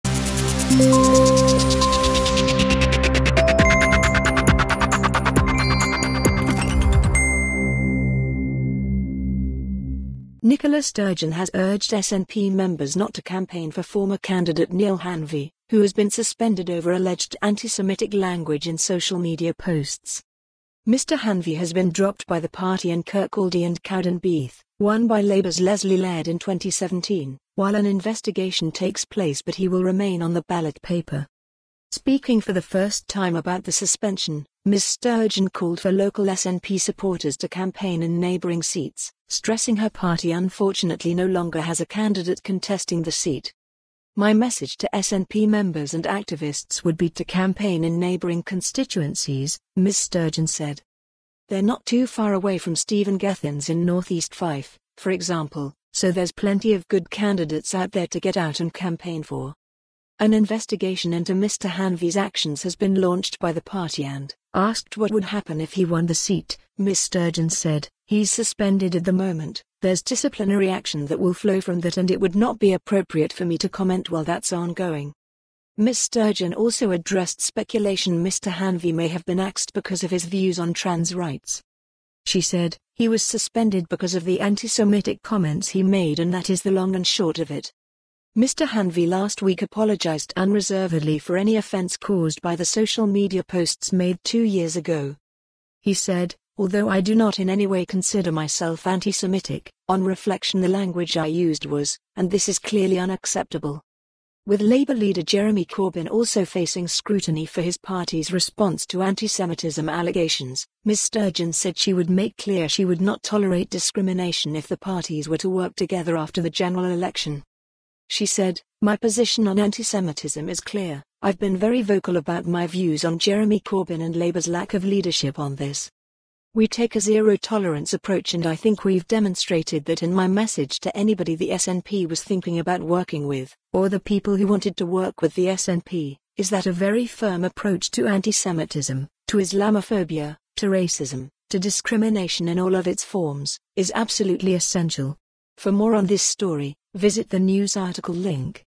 Nicola Sturgeon urges SNP members not to campaign for axed Fife candidate
Nicola Sturgeon has urged SNP members not to campaign for former candidate Neale Hanvey, who has been suspended over alleged anti-Semitic language in social media posts.

Mr Hanvey has been dropped by the party in Kirkcaldy and Cowdenbeath – won by Labour's Lesley Laird in 2017 – while an investigation takes place but he will remain on the ballot paper.

Speaking for the first time about the suspension, Ms Sturgeon called for local SNP supporters to campaign in neighbouring seats, stressing her party "unfortunately" no longer has a candidate contesting the seat.

"My message to SNP members and activists would be to campaign in neighbouring constituencies," Ms Sturgeon said.

"They're not too far away from Stephen Gethins in North East Fife, for example, so there's plenty of good candidates out there to get out and campaign for."

An investigation into Mr Hanvey's actions has been launched by the party and, asked what would happen if he won the seat, Ms Sturgeon said: "He's suspended at the moment, there's disciplinary action that will flow from that and it would not be appropriate for me to comment while that's ongoing."

Ms Sturgeon also addressed speculation Mr Hanvey may have been axed because of his views on trans rights.

She said: "He was suspended because of the anti-Semitic comments he made and that is the long and short of it."

Mr Hanvey last week apologised "unreservedly" for any offence caused by the social media posts made two years ago.

He said: "Although I do not in any way consider myself anti-Semitic, on reflection the language I used was, and this is clearly unacceptable."

With Labour leader Jeremy Corbyn also facing scrutiny for his party's response to anti-Semitism allegations, Ms Sturgeon said she would make clear she